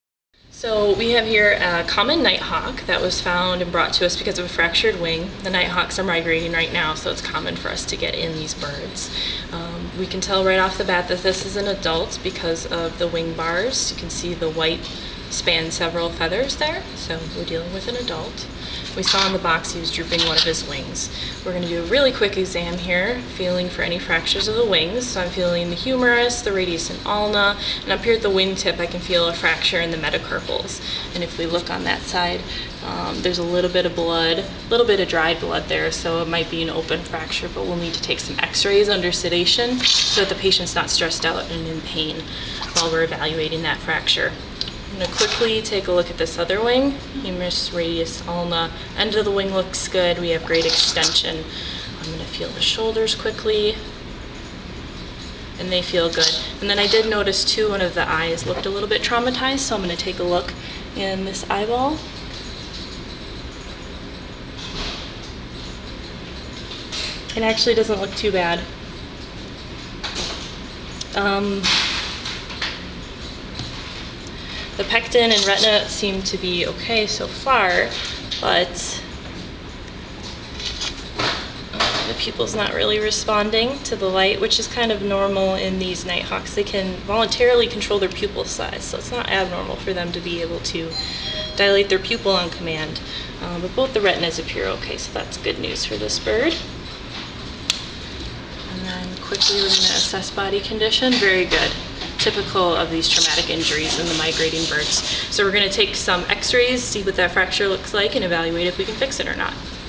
Common Nighthawk Exam
Common Nighthawks are in their fall migration (Aug. 29). We're admitting several a week at this time of year, most with fractured wings.

This video, as with all our exam videos, is shot during the actual exam process as to not cause further stress or harm to our patients. (we don't simply do multiple exams for filming purposes)